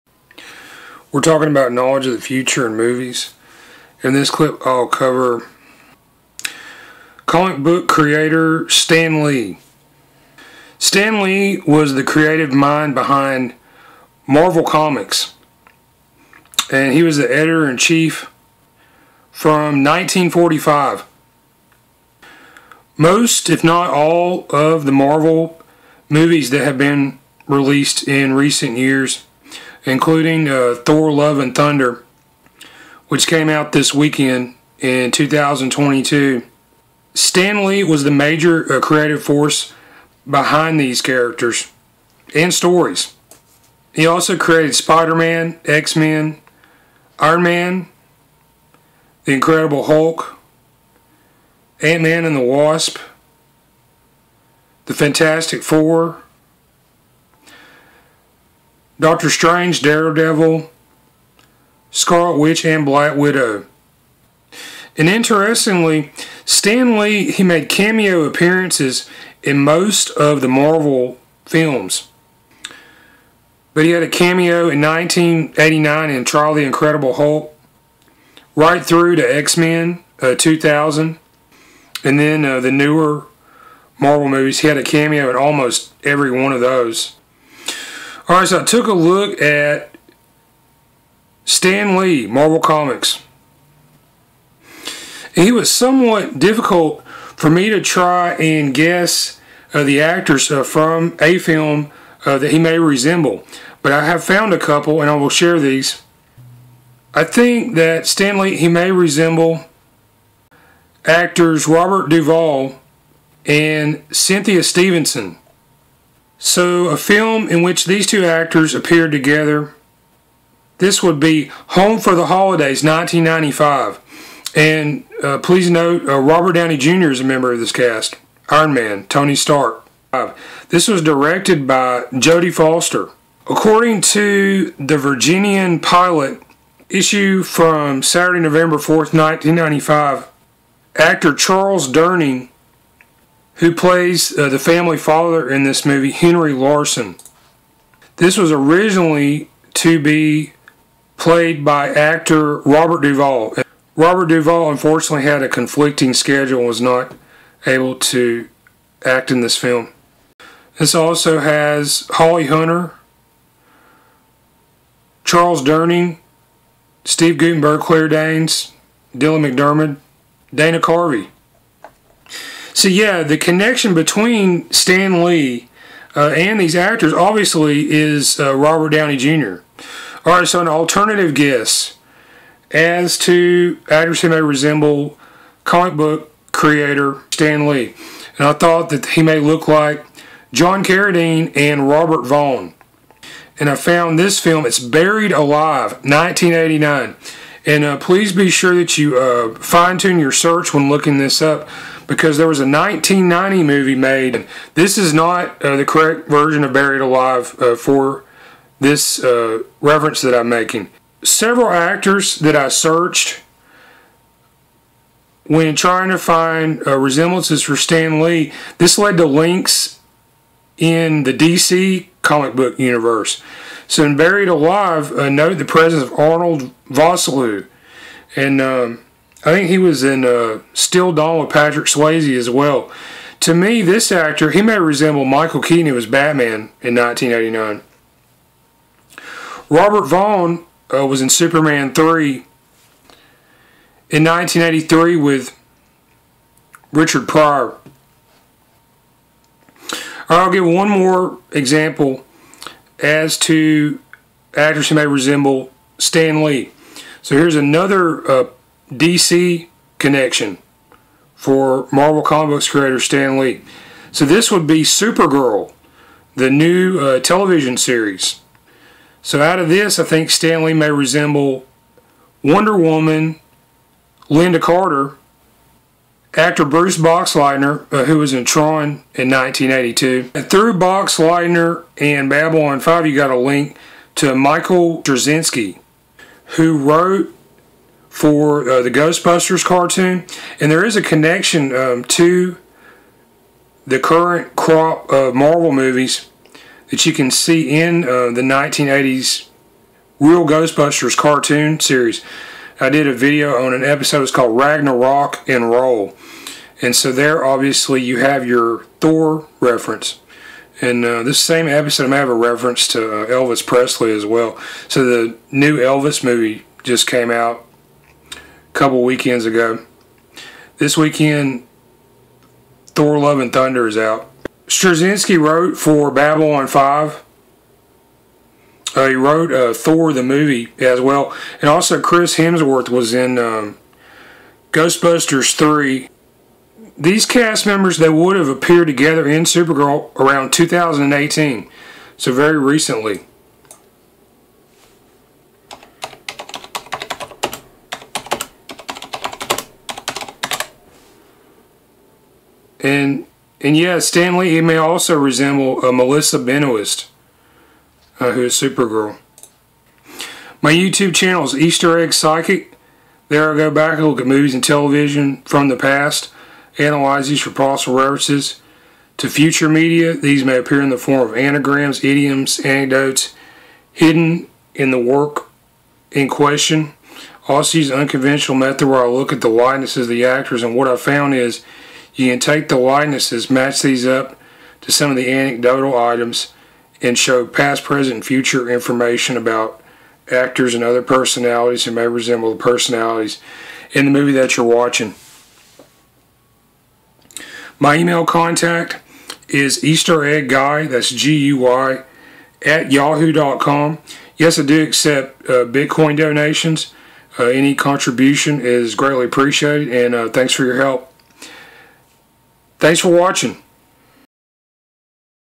Does Marvel's Stan Lee Resemble Robert Duvall & Cynthia Stevenson? - Home for the Holidays (1995)
Marvel Comic Book creator, Stan Lee, may resemble actors Robert Duvall & Cynthia Stevenson who were both involved with the 1995 film, Home For The Holidays. Duvall was scheduled to play the family father in this movie, later replaced by actor Charles Durning. This was directed by Jodie Foster with Iron man / Tony Stark: Robert Downey Jr., Holly Hunter, Claire Danes, Dylan McDermott & Steve Guttenberg.

Stan Lee may also resemble John Carradine and Robert Vaughn of Buried Alive (1989). This possible combination shows links to the DC universe. Vaughn was in Superman 3 in 1983 with Richard Pryor. Arnold Vosloo may resemble Batm,an (1989) actor Michael Keaton.

I also thought Stan Lee may resemble Supergirl TV actress Melissa Benoist, Wonder Woman Lynda Carter and Tron actor Bruce Boxleitner from a 2018 episode.